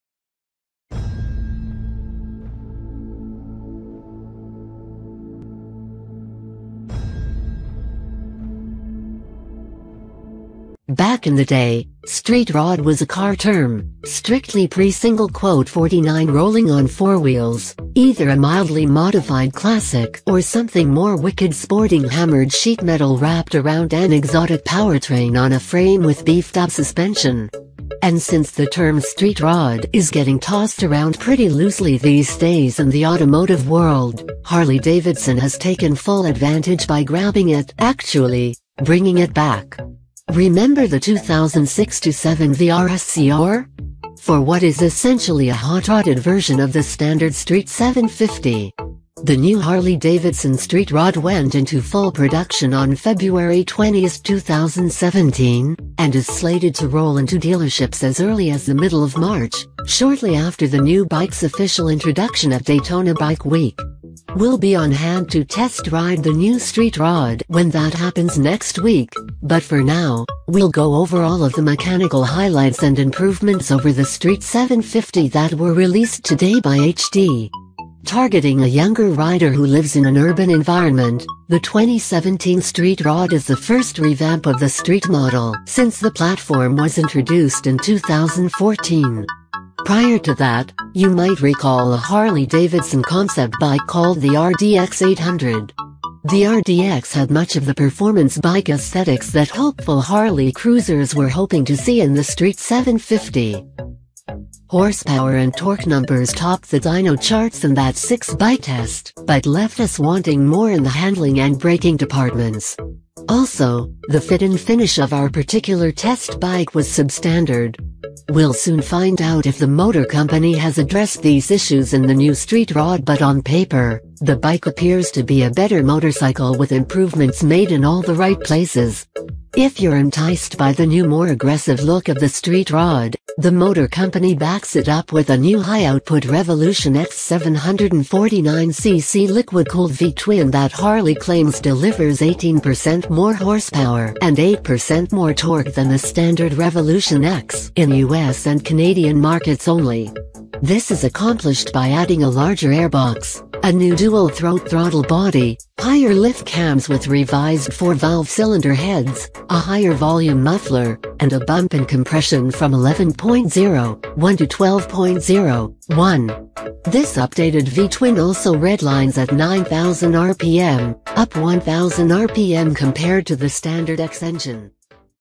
2017 Harley-Davidson Street Rod a Higher-Output Revolution X Engine In a Revised Chassis
In this video contains slide show photo and the latest news about motorcycle. Hope this video will help you and more people to get information about motorcycle.